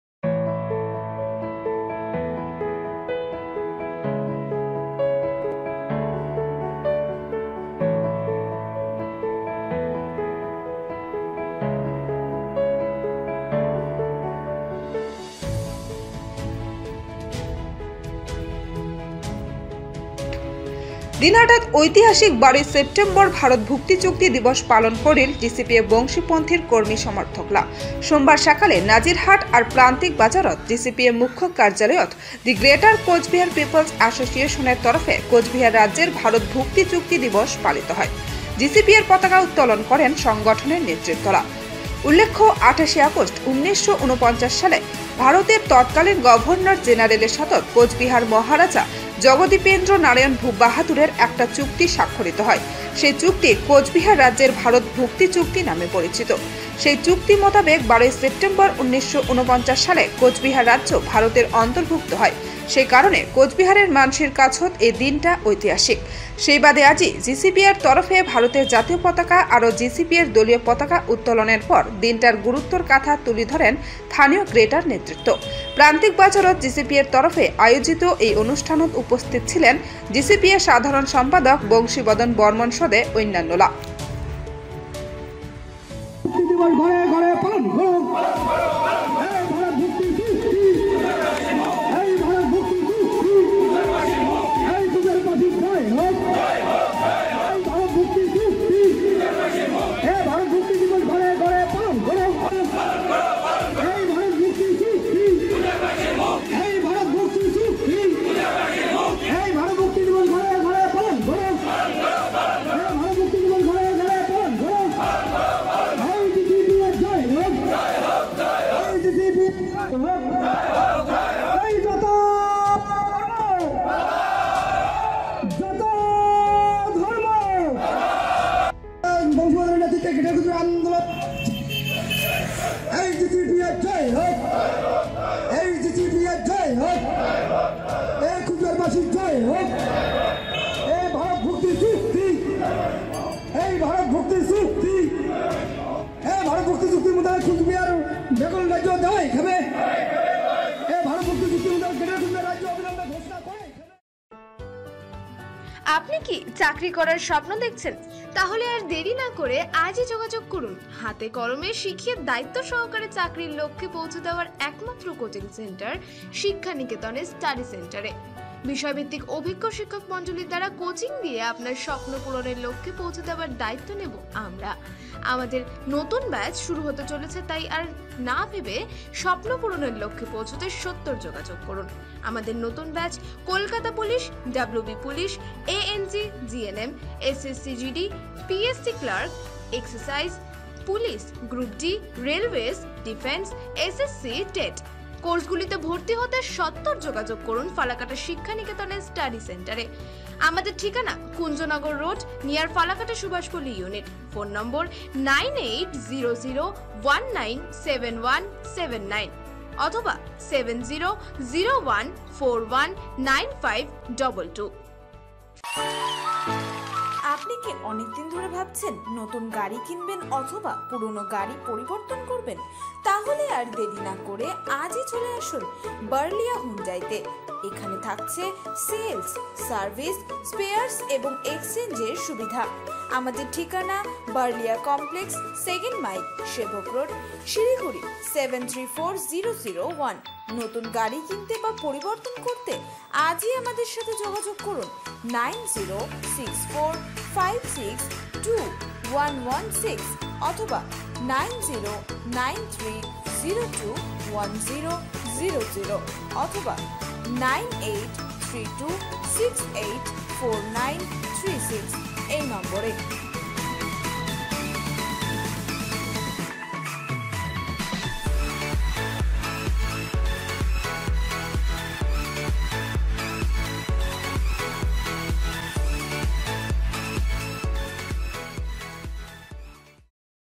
ভুক্তি চুক্তি দিবস পালন করিল GCPA বংশী পন্থীর কর্মী সমর্থকলা
দিনহাটাত ঐতিহাসিক ১২ সেপ্টেম্বর ভারত ভুক্তি চুক্তি দিবস পালন করিল GCPA বংশী পন্থীর কর্মী সমর্থকলা

রিপোর্ট : দিনহাটা, রাহুল দেব বর্মন